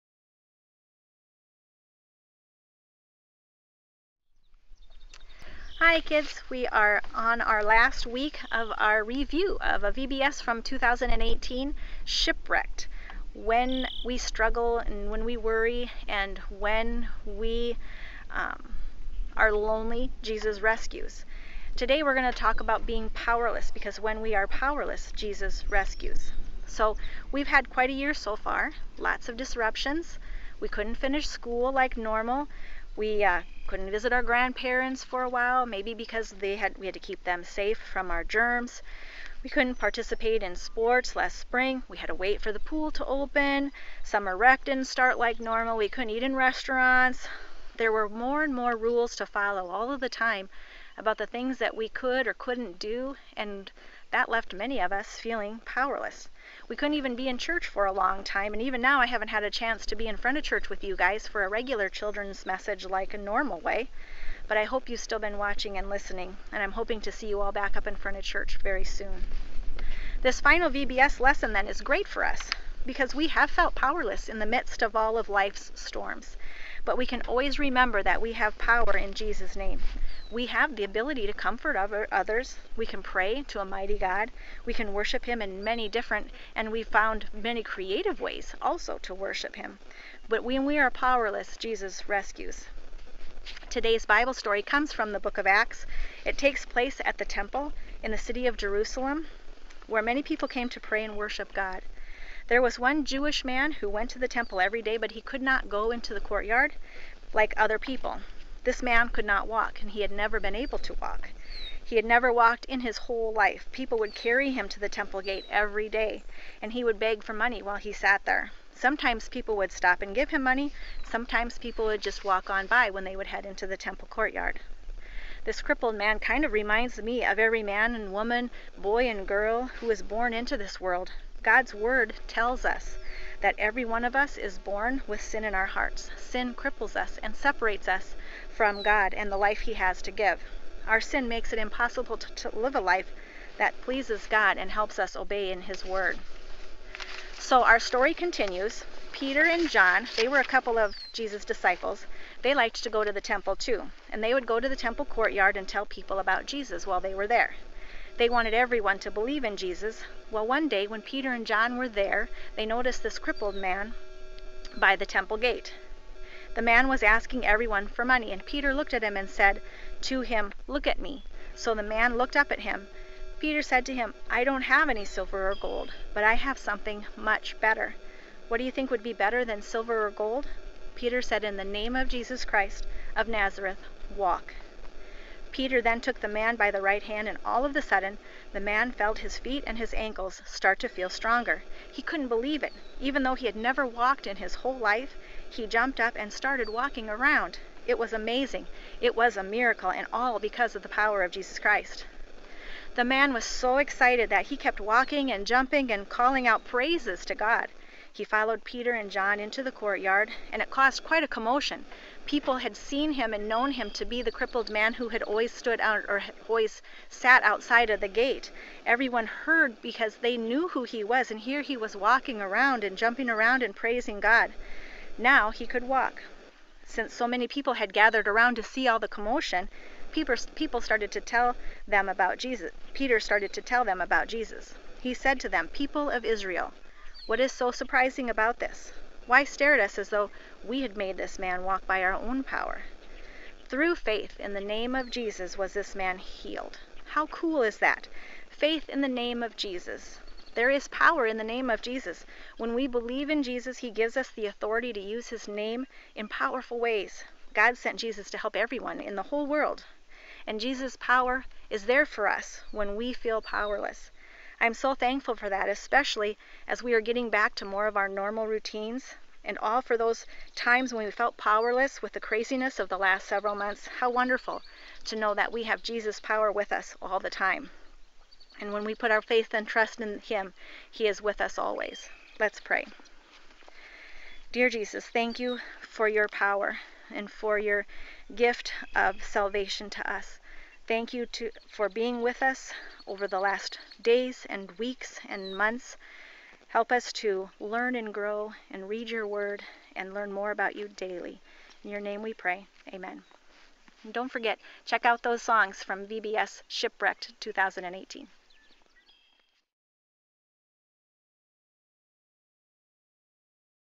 FRC Kids @ Home: Shipwrecked VBS Week 4
We are in our final week of reviewing themes from our VBS theme for 2018.